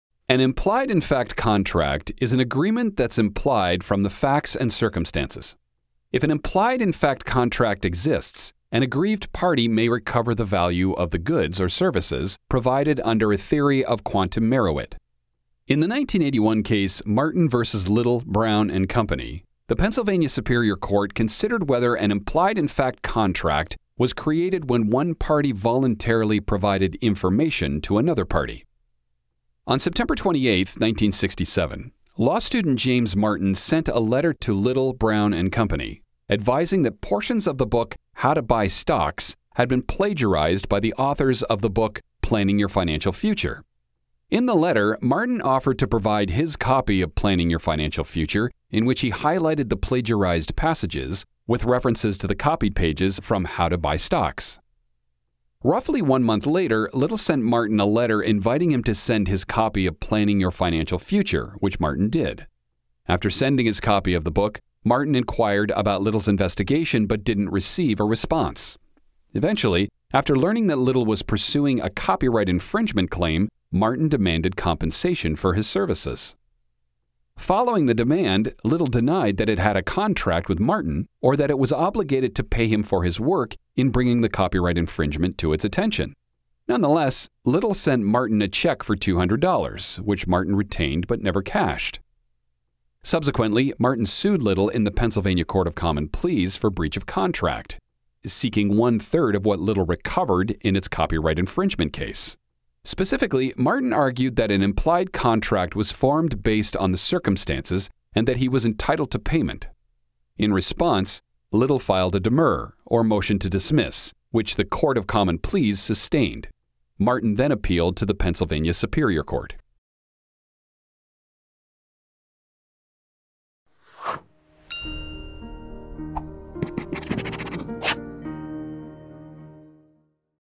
Martin v. Little, Brown and Company Case Brief Summary | Law Case Explained
Martin v. Little, Brown & Co. | 450 A.2d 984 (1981)

An implied-in-fact contract is an agreement that’s implied from the facts and circumstances.  If an implied-in-fact contract exists, an aggrieved party may recover the value of the goods or services provided under a theory of quantum meruit.  In the 1981 case Martin versus Little, Brown and Company, the Pennsylvania Superior Court considered whether an implied-in-fact contract was created when one party voluntarily provided information to another party.

On September 28th, 1967, law student James Martin sent a letter to Little, Brown and Company advising that portions of the book How to Buy Stocks had been plagiarized by the authors of the book Planning Your Financial Future. In the letter, Martin offered to provide his copy of Planning Your Financial Future, in which he highlighted the plagiarized passages with references to the copied pages from How to Buy Stocks.

Roughly one month later, Little sent Martin a letter inviting him to send his copy of Planning Your Financial Future, which Martin did. After sending his copy of the book, Martin inquired about Little’s investigation but didn’t receive a response. Eventually, after learning that Little was pursuing a copyright-infringement claim, Martin demanded compensation for his services.

Following the demand, Little denied that it had a contract with Martin or that it was obligated to pay him for his work in bringing the copyright infringement to its attention. Nonetheless, Little sent Martin a check for two hundred dollars, which Martin retained but never cashed.

Subsequently, Martin sued Little in the Pennsylvania Court of Common Pleas for breach of contract, seeking one-third of what Little recovered in its copyright-infringement case. Specifically, Martin argued that an implied contract was formed based on the circumstances, and that he was entitled to payment. In response, Little filed a demurrer, or motion to dismiss, which the court of common pleas sustained. Martin then appealed to the Pennsylvania Superior Court.

Want more details on this case? Get the rule of law, issues, holding and reasonings, and more case facts

Have Questions about this Case?
Submit your questions and get answers from a real attorney